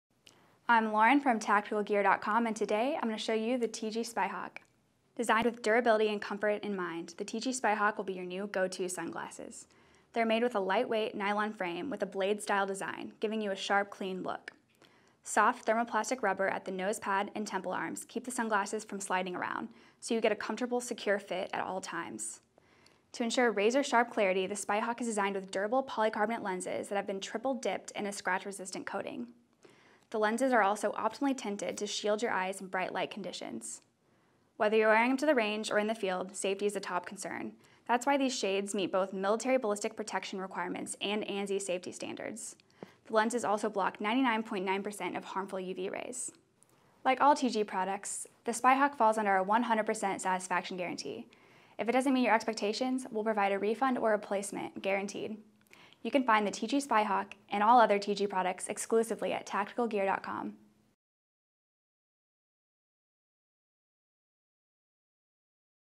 TG Spyhawk at TacticalGear.com
With a clean, authentic design, the TG Spyhawk makes simple look good. The blade-style construction gives a streamlined look that functions virtually everywhere, from patrol to the field to the shooting range. When safety is a top concern, these optics have you covered. They meet military ballistic standards and are ANSI Z87.1 safety rated for maximum protection. Acquire targets and engage with greater precision and speed with the Spyhawk’s razor-sharp clarity. The tough polycarbonate lenses are triple-dipped for scratch resistance and sport an optimized tint for bright-light conditions. In addition, the soft thermoplastic rubber nose pads and temple arms allow the light nylon frames to sit comfortably and securely all-day long. Sport originality, functionality and a smooth look with the TG Spyhawk.